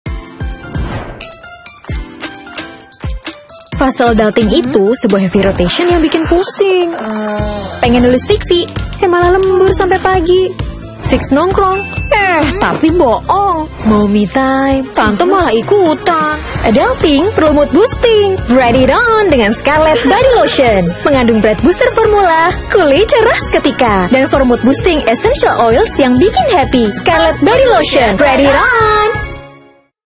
Scarlett NEW Body Lotion
Formula terbaru dari Scarlett Body Lotion bikin good mood seharian dan cerah seketika!